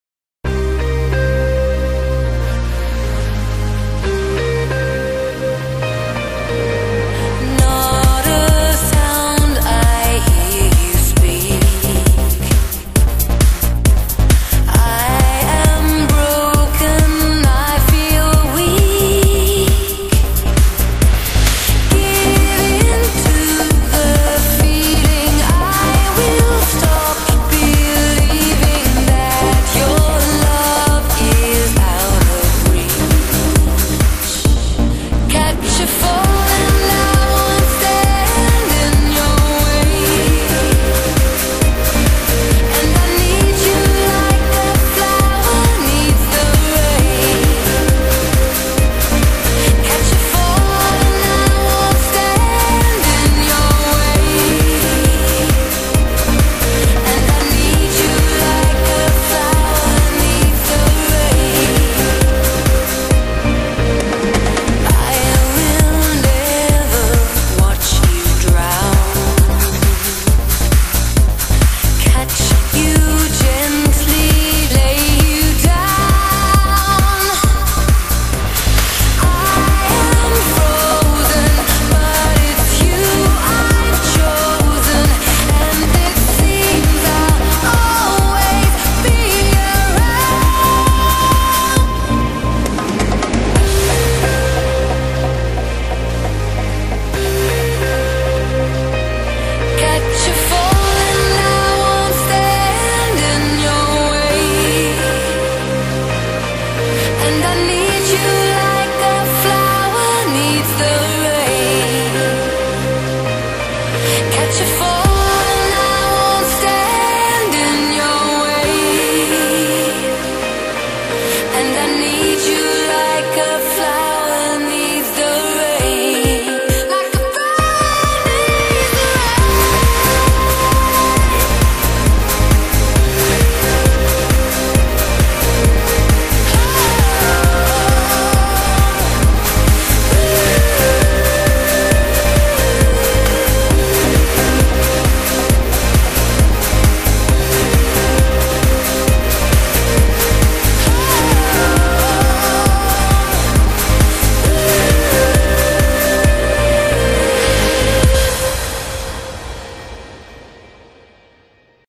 Krzysztof Halicki outdoor 3D
outdoor 3D from Poland ! Model katana 1,25m, mzk octava receiver, motor AXI 2814/20 + 12x6 prop( 4s 2250mah wow RC - flight time 15min ). Good watchnig !